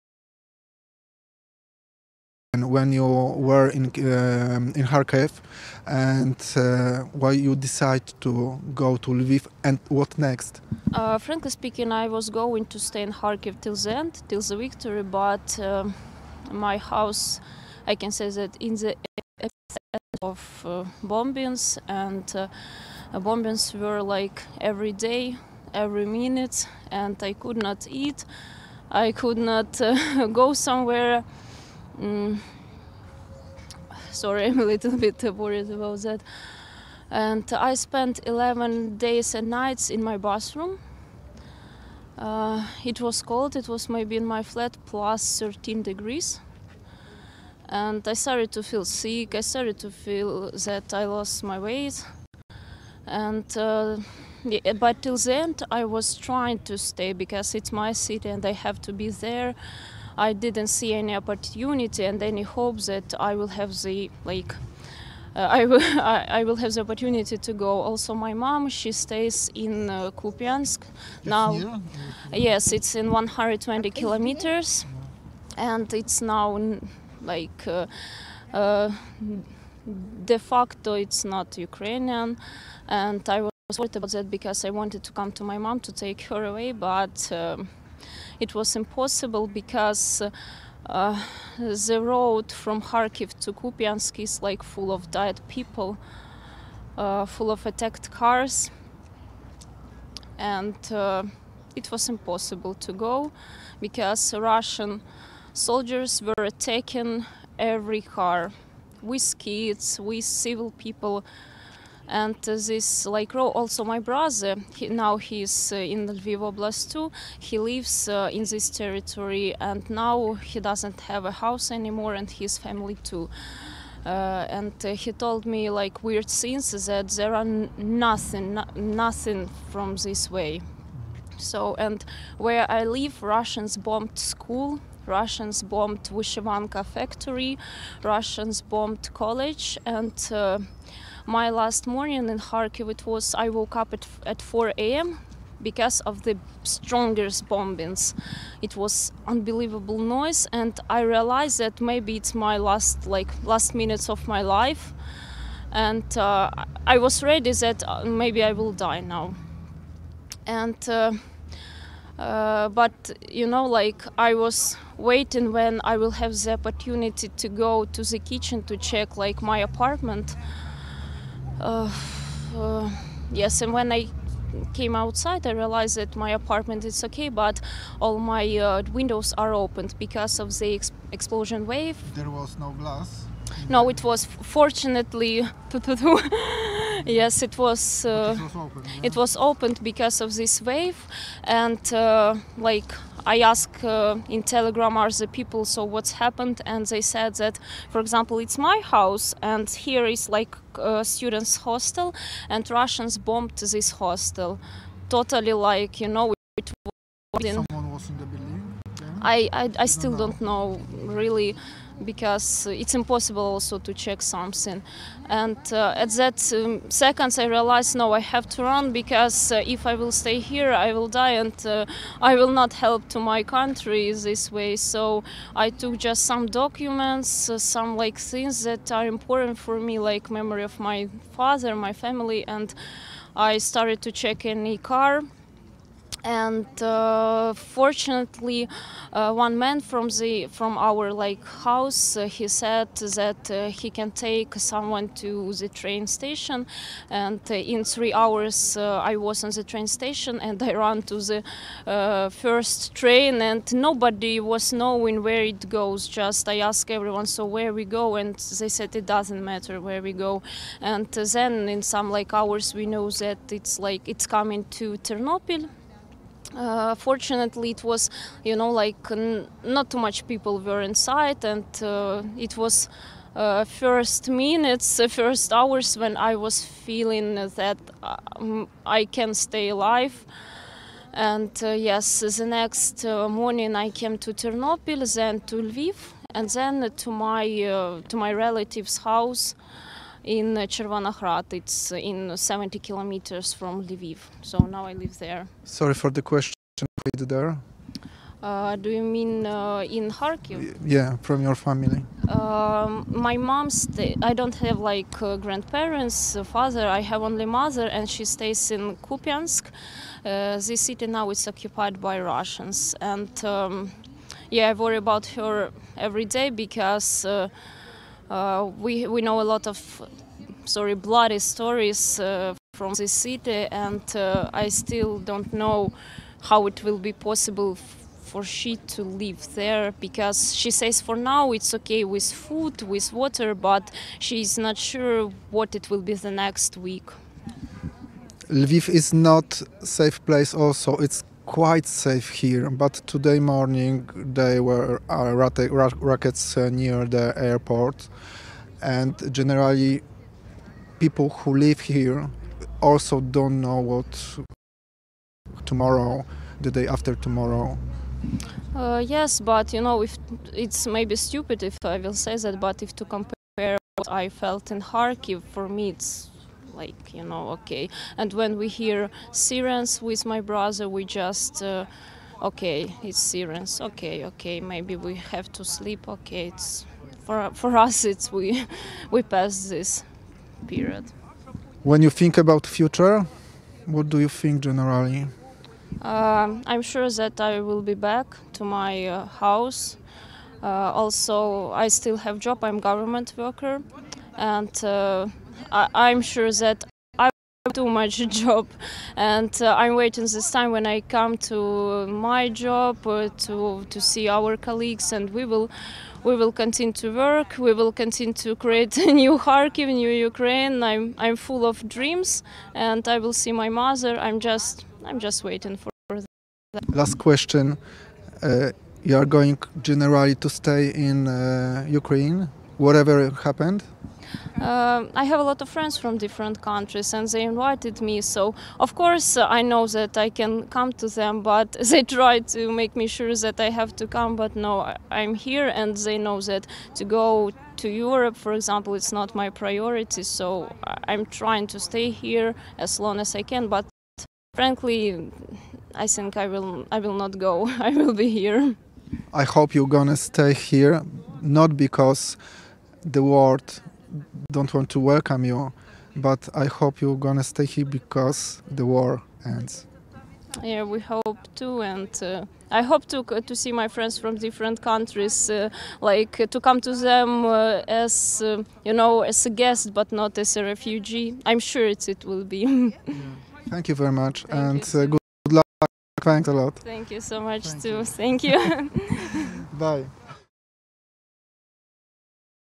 "Byłam gotowa na śmierć". Mieszkanka Charkowa o swojej ucieczce z bombardowanego miasta
Anastasiia z Charkowa opowiedziała naszemu reporterowi o traumatycznych przeżyciach, jakich doświadczyła podczas bombardowań jej rodzinnego miasta. Chowając się przed bombami, spędziła 11 dni w swojej łazience. W końcu udało jej się wydostać pociągiem i dotrzeć w okolice Lwowa.